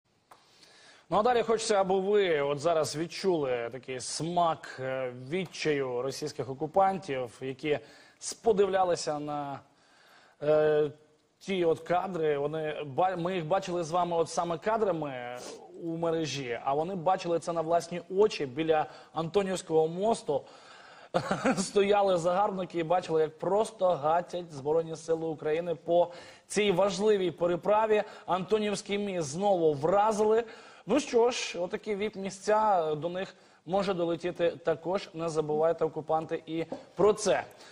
Військові РФ спостерігають з понтонів, як ЗСУ добивають Антонівський міст
Українські військові, 4 вересня, вчергове завдали ударів по Антонівському мосту на Херсонщині.